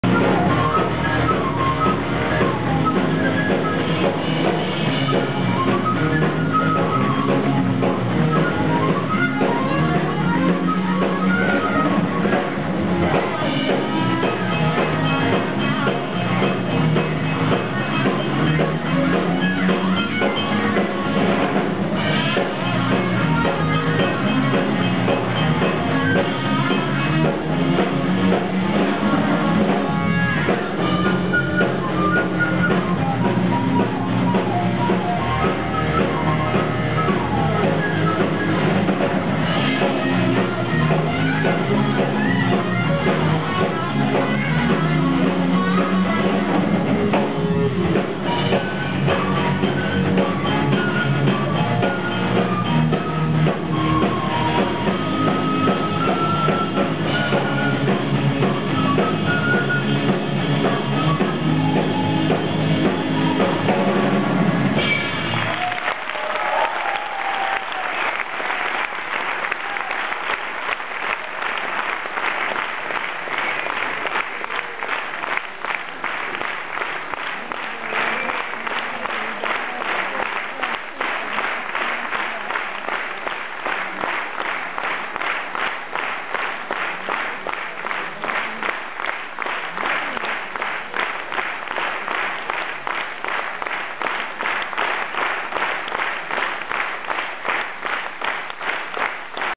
Horgas Eszter és a Class Jazz Band 1.
A pápai Jókai Mór Művelődési és Szabadidő Központban 2016. január 22-én, a magyar kultúra napján léptek fel - nagy sikert aratva!